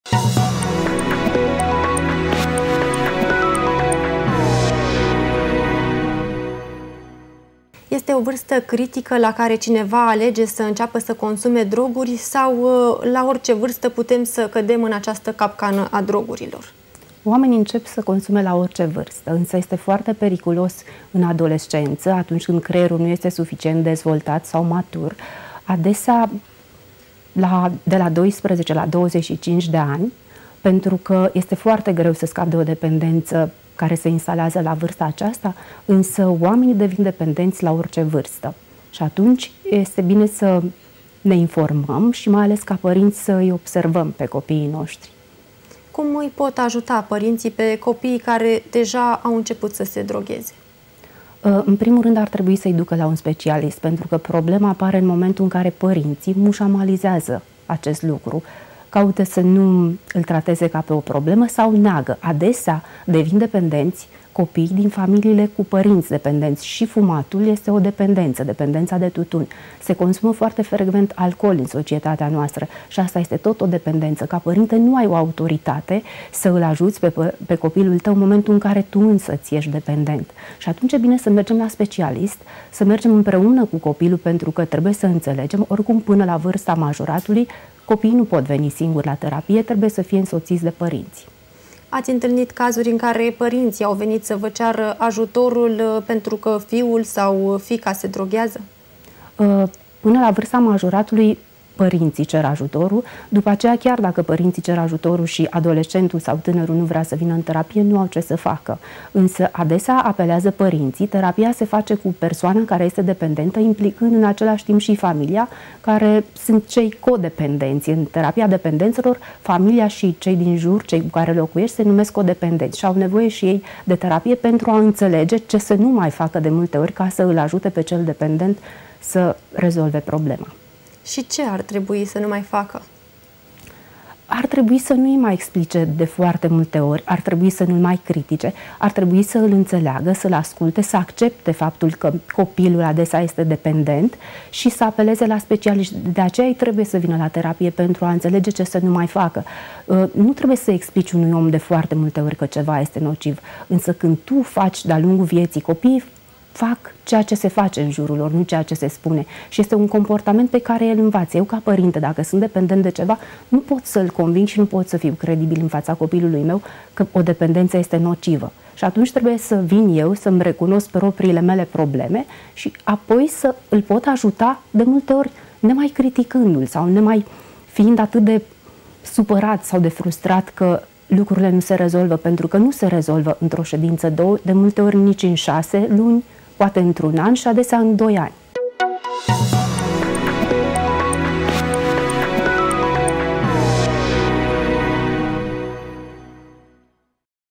DROGURILE UCID! TRĂIEȘTE VIAȚA!
Vă prezentăm un nou material din cadrul campaniei "Drogurile ucid! Trăiește viața!", o campanie de informare, educare și conștientizare a riscurilor medicale și sociale ale consumului de droguri. Prin această campanie ne dorim ca adolescenții și tinerii să adopte un stil de viață sănătos. Campania se adresează și comunității şi subliniază importanța unei atitudini responsabile din partea adulților: părinți, profesori sau reprezentanți ai autorităților cu atribuții în domeniu, pentru a descuraja consumul de droguri în rândul tinerilor. Toate statisticile arată o creștere a consumului de droguri în ultimii ani în România. Aproape 11% dintre persoanele care au între 15 și 64 de ani au consumat cel puțin o dată droguri, arată un raport al Agenției Naționale Antidrog. Aflăm mai multe detalii de la psihoterapeutul Ana Dobre.